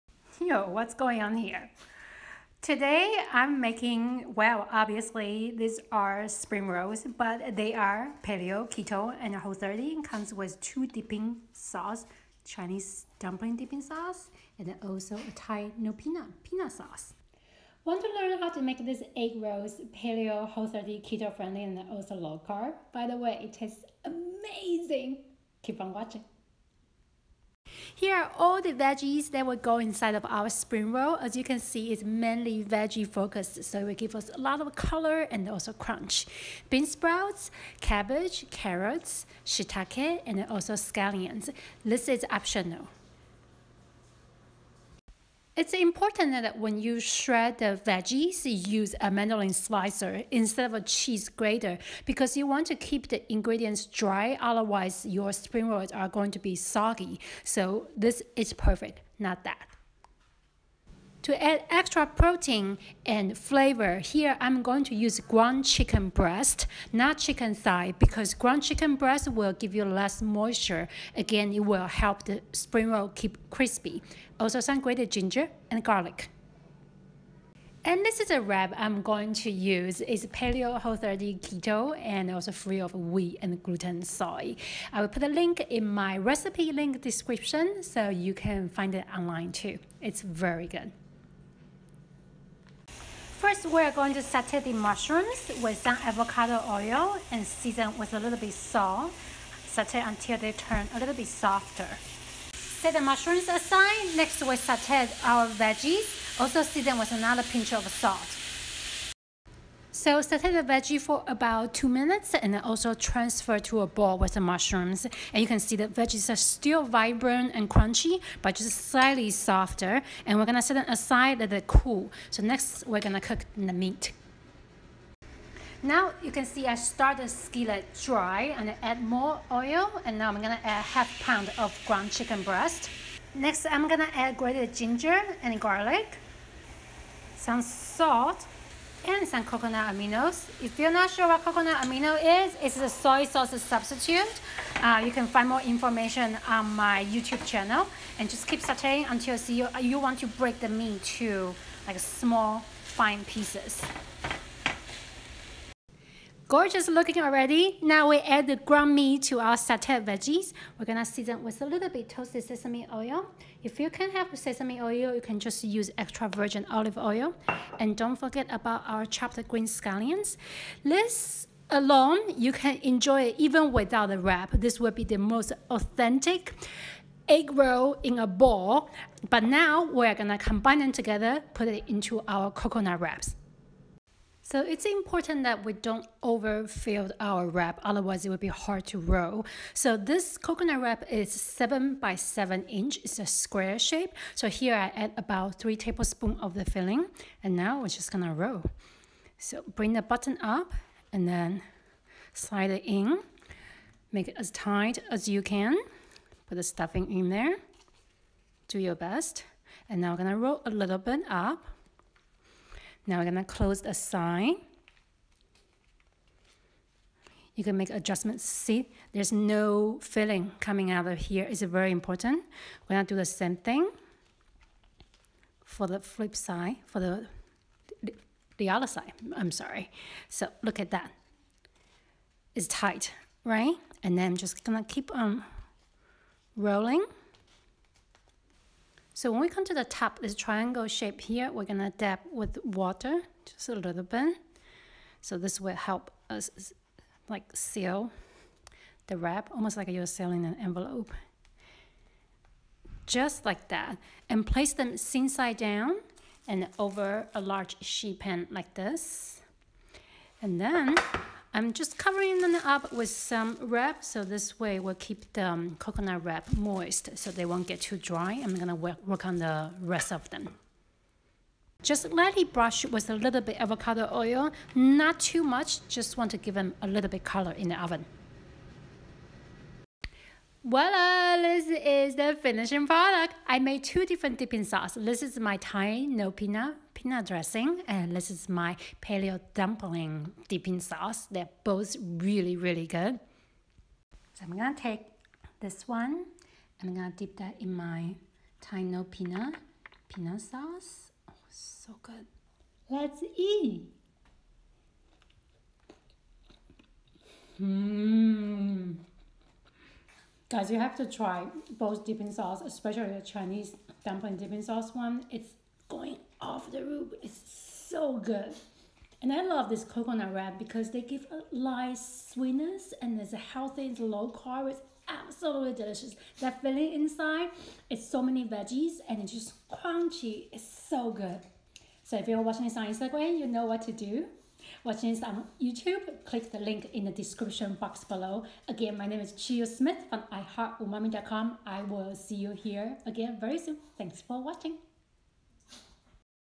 Paleo Egg Rolls Without A Bowl (low carb/gluten-free baked egg rolls)
Paleo Egg Rolls Without A Bowl (low carb/gluten-free baked egg rolls)//Hey friends! This week to share one of my favorite low carb appetizer - Paleo Egg Rolls! These low carb egg rolls are filled with shredded cabbage, sweet carrots, bean sprouts, and all the other yummy stuff and wrapped in coconut wraps. We then bake these low carb keto egg rolls in the oven until they are golden crispy! This recipe is a healthier take on the traditional Chinese spring roll recipe and the filling is so delicious!

You can also make it without the coconut wraps for paleo egg rolls in a bowl. These healthy egg rolls will make the best gluten-free egg rolls for parties and family gathering.

Find more super yummy Paleo, Whole30, and Keto recipes for your weekly healthy meal prep